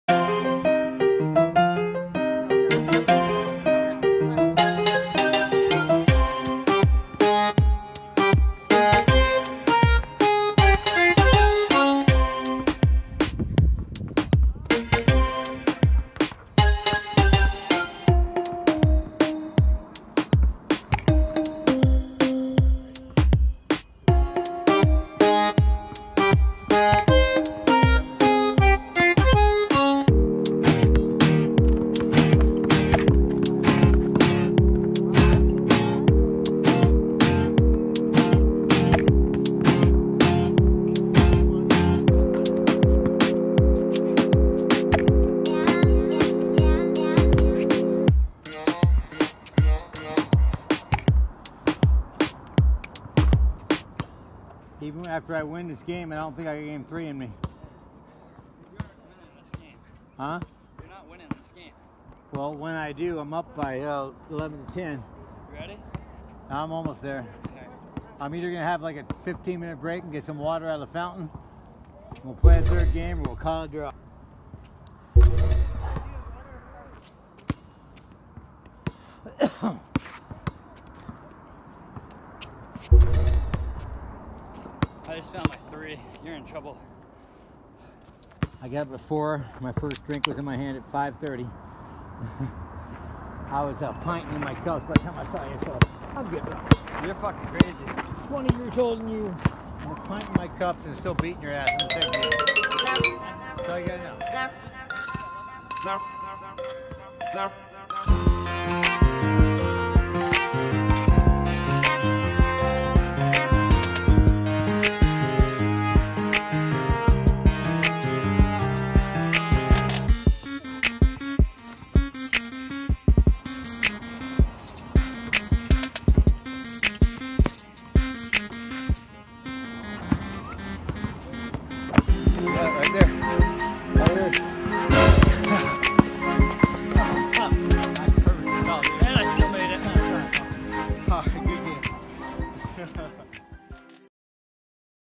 Game 2 Bernie vs Boback
Bernie aims to finish the old man once and for all but the old dog Boback still has some fight in him